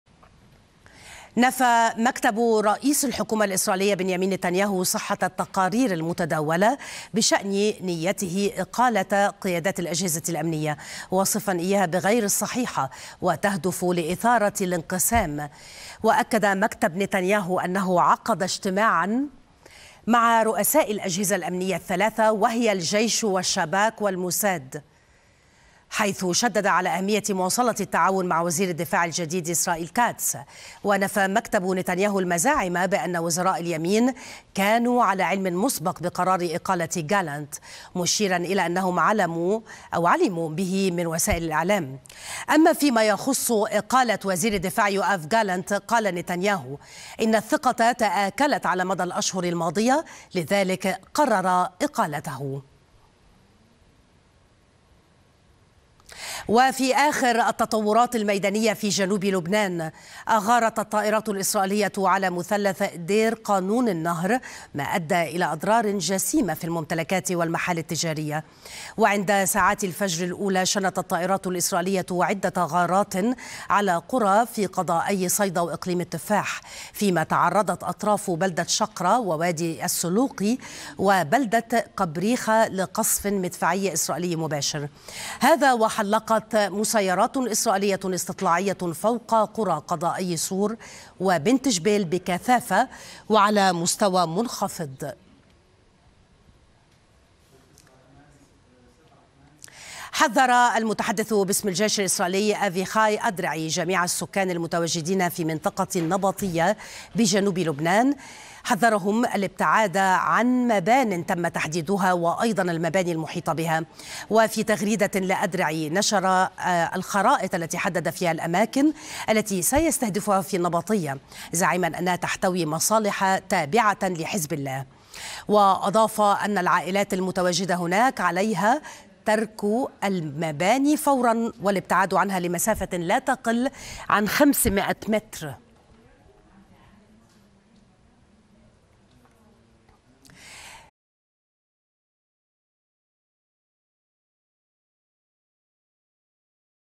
مكتب نتنياهو ينفي نيته إقالة قادة أجهزة أمنية في إسرائيل
مكتب نتنياهو: لا صحة للتقارير المتداولة بشأن وجود نية لإقالة قيادات بالأجهزة الأمنية في إسرائيل